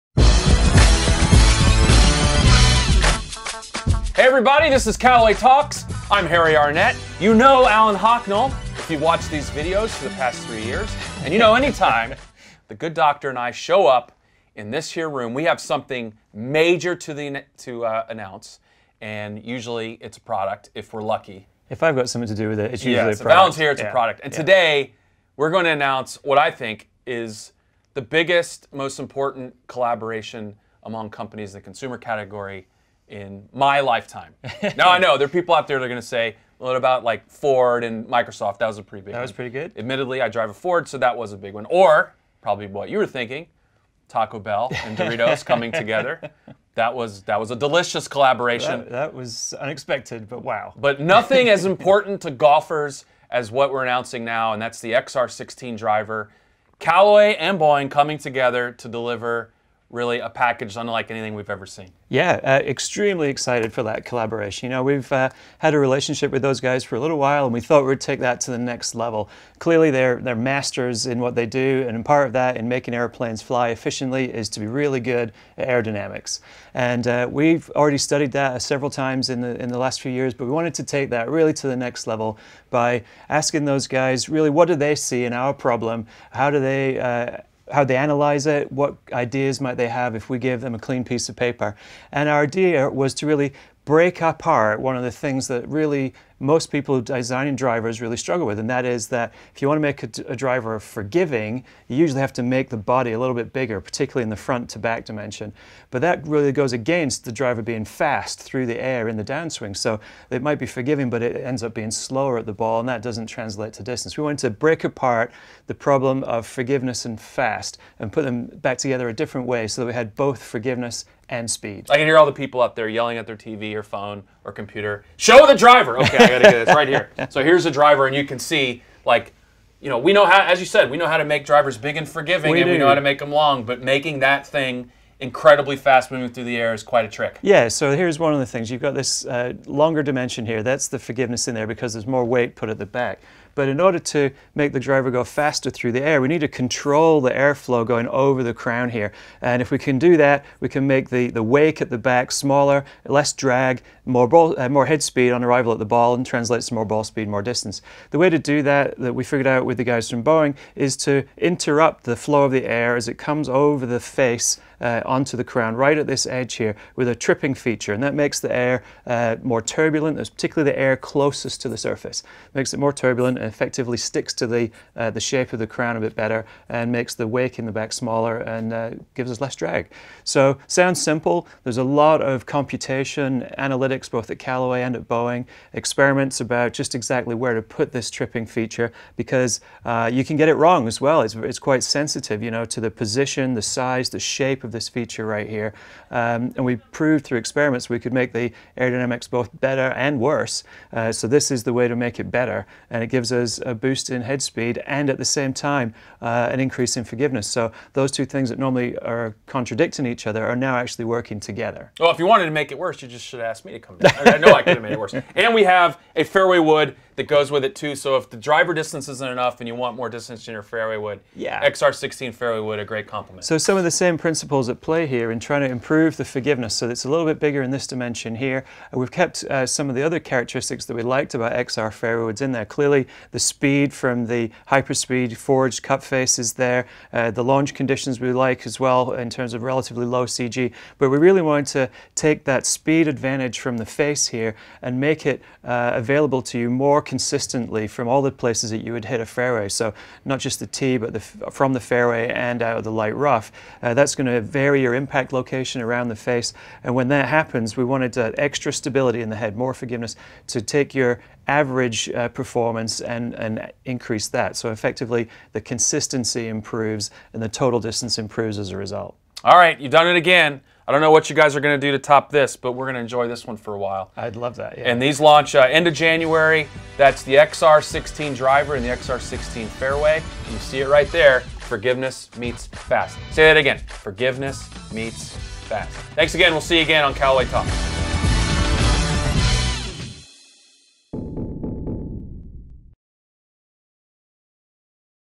Are The New XR 16 Drivers & Fairway Woods Really That Forgiving?
Alan Hocknell, VP of R&D at Callaway, tells us how the new line of XR Drivers & Fairway Woods differ from anything else we've ever made before.